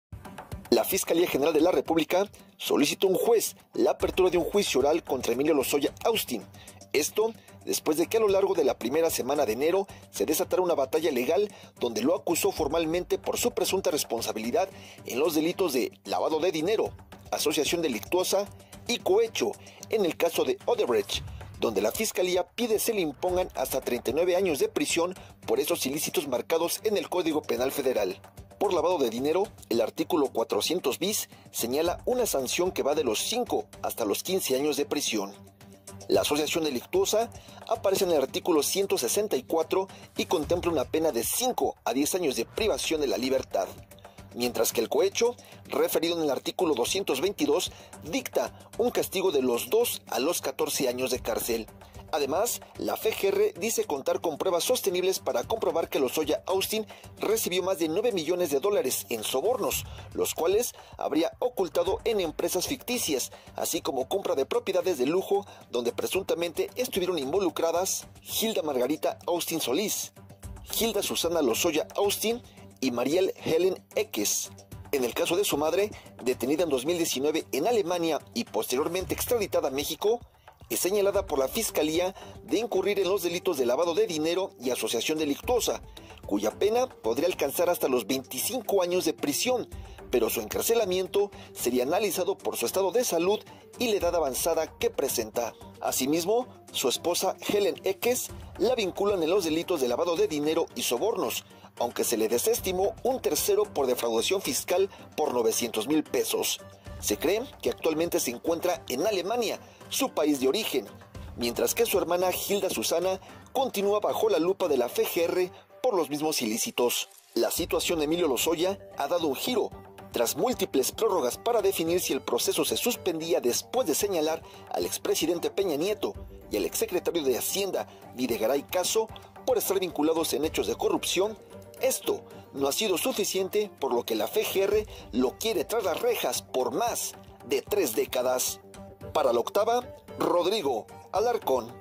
Los DELITOS por los que LOZOYA podría pasar 39 AÑOS en la CÁRCEL
La FGR quiere que Emilio Lozoya pase 39 años en la cárcel por el caso Odebrecht y asegura que tiene pruebas de que Lozoya recibió más de 9 millones de dólares.